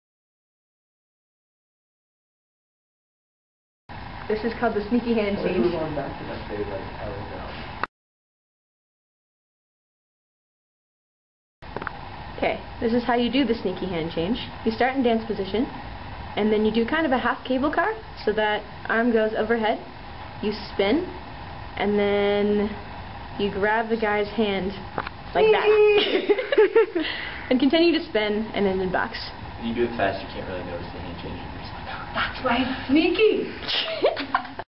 Sneaky Hand Change Instructional
Instructional video for the country western dance hand change that allows one to go from dance position to box position with style.